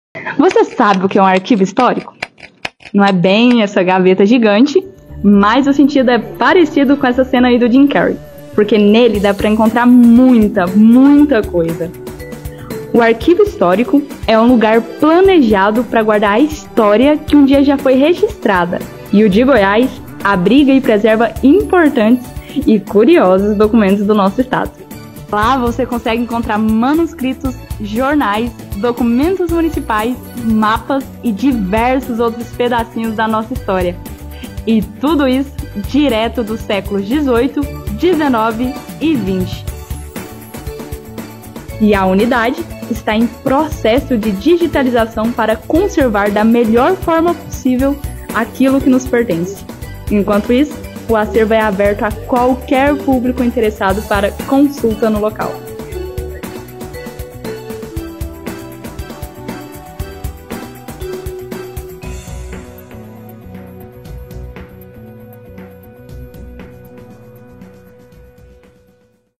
O QUE É UM ARQUIVO HISTÓRICO?
Vem com a gente conhecer um pouco sobre o Arquivo Histórico Estadual!

🗃️ A unidade fica aberta de segunda a sexta-feira, das 8h às 17h. A visita e a consulta a todo o acervo é GRATUITA. Vem ver!